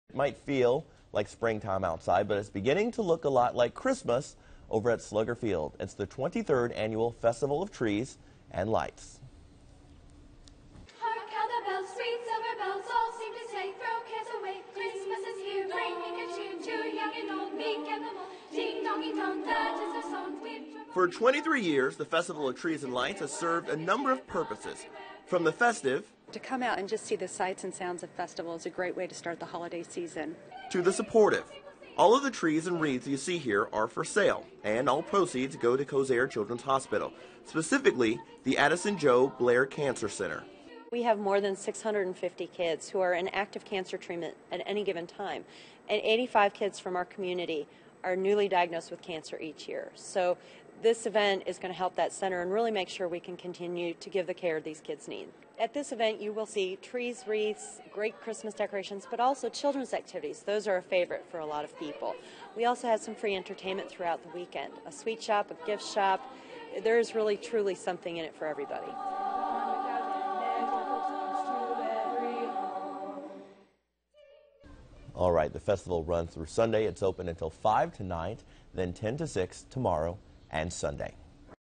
Festival of Trees and Lights brings holiday spirit to town
It's beginning to look a lot a lot like Christmas at Slugger Field for the 23rd annual Festival of Trees and Lights.